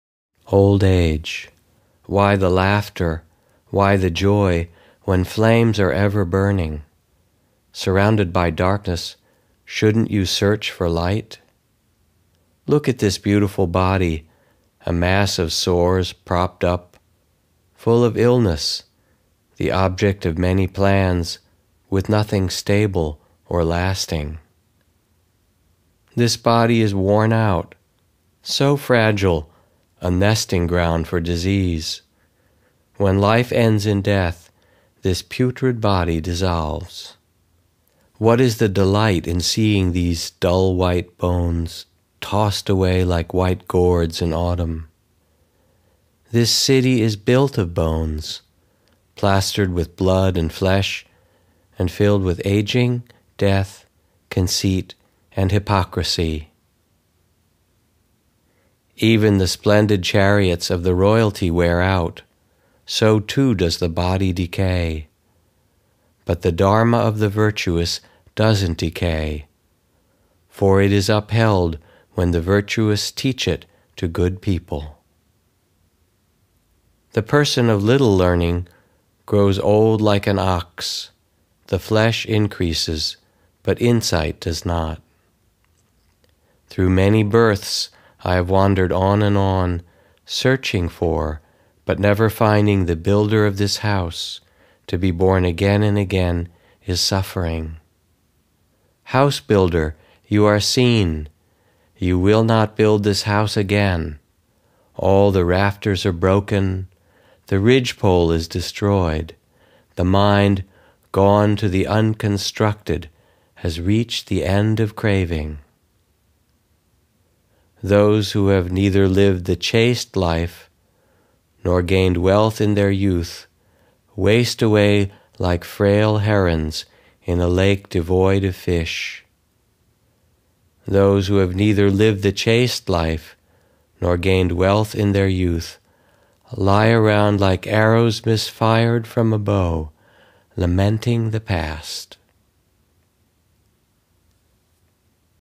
The Dhammapada 法句經 -- "Old Age" -- "晚年" 11 of 26
Why the laughter, why the joy,
When flames are ever burning?
Surrounded by darkness,
Shouldn't you search for light?
(146)

Look at this beautified body:
A mass of sores propped up,
Full of illness, (the object) of many plans,
With nothing stable or lasting.
(147)

This body is worn out—
So fragile, a nesting ground for disease.
When life ends in death,
This putrid body dissolves.
(148)

What is the delight
In seeing these dull-white bones
Tossed away
Like white gourds in autumn?
(149)

This city is built of bones,
Plastered with blood and flesh,
And filled with
Aging, death, conceit, and hypocrisy.
(150)

Even the splendid chariots of the royalty wear out.
So too does the body decay.
But the Dharma of the virtuous doesn't decay
[For it is uphelp when] the virtuous teach [it] to
good people.
(151)

The person of little learning
Grow old like an ox:
The flesh increases,
But insight does not.
(152)

Through many births
I have wandered on and on,
Searching for, but never finding,
The builder of [this] house.
To be born again and again is suffering.
House-builder, you are seen!
You will not build a house again!
All the rafters are broken,
The ridgepole destroyed;
The mind, gone to the Unconstructed,
Has reached the end of craving!
(153-154)

Those who have neither lived the chaste life
Not gained wealth in their youth
Waste away like frail herons
In a lake devoid of fish.
(155)

Nor gained wealth in their youth
Lie around like [arrows misfired] from a bow,
Lamenting the past.
(156)

Translated by Gil Fronsdal
Read by Jack Kornfield

My purpose for up loading this on to YouTube is to share this
with the world and for those who search for it.

My deepest sincere Thank You
to all for allowing me to accomplish this task in life.
May all be bless with merits ~

Amitabha
阿彌陀佛

Namasté ~